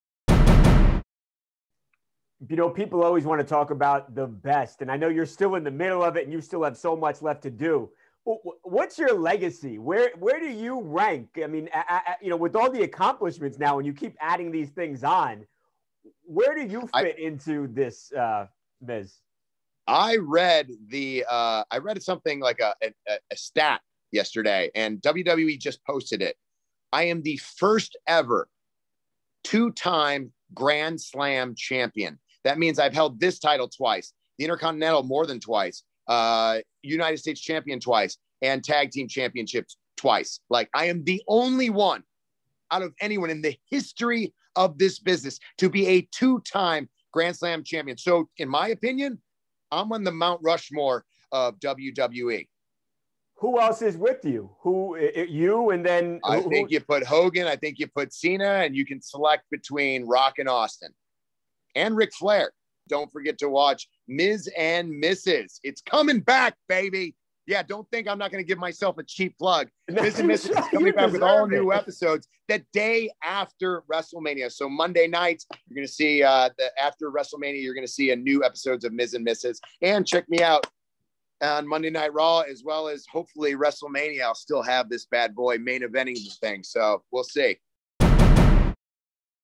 The Miz Says He Belongs on Mt. Rushmore of WWE With Hulk Hogan & John Cena | TMZ Sports
If the WWE decides to carve 4 heads into the side of a giant mountain to honor the greatest pro wrestlers in company history, The Miz says his face better be on there.